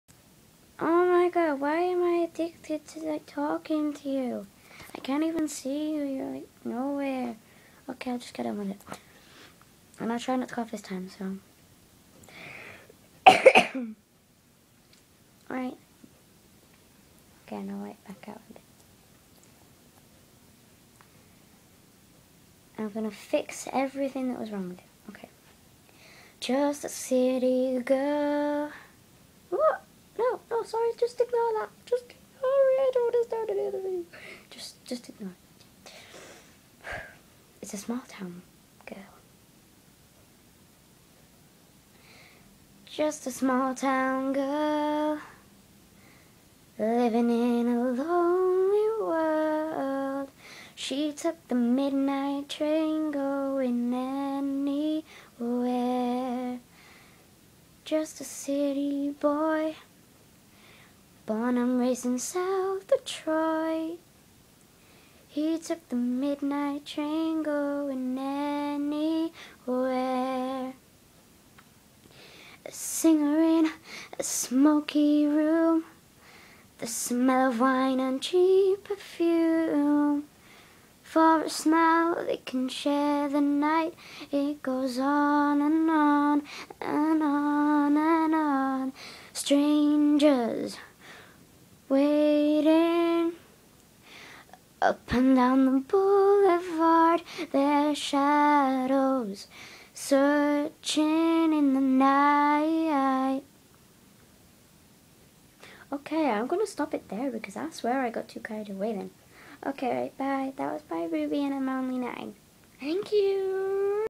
ShowUrTalent - Giving It a go!!!
Just me singing!. Uploaded by MiniMe on Show Your Talent.
To rate this video please download the Show Your Talent app on the Apple App Store